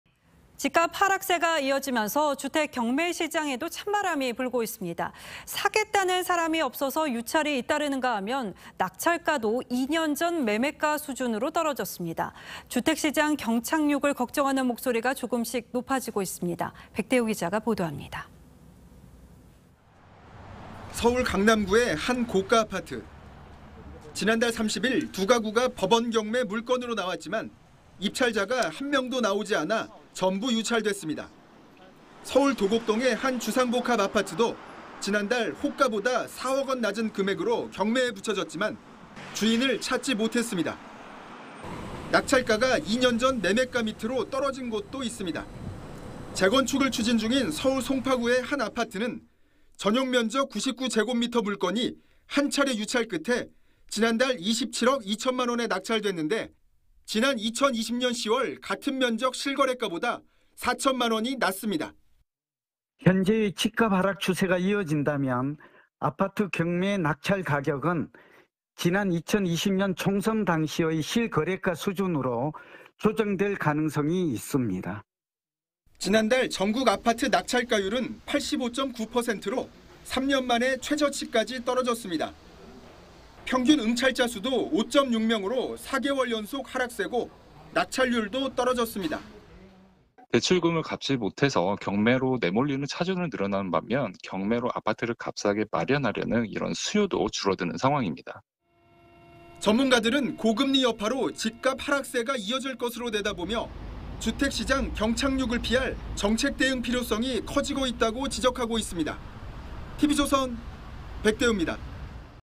주택 경매도 찬바람…잇단 유찰에 낙찰가도 2년전 수준으로
집값 하락세가 이어지면서 주택 경매 시장에도 찬바람이 불고 있습니다. 사겠다는 사람이 없어 유찰이 잇따르는가 하면, 낙찰가도 2년 전 매매가 수준으로 떨어졌습니다. 주택 시장 경착륙을 걱정하는 목소리가 조금씩 높아지고 있습니다.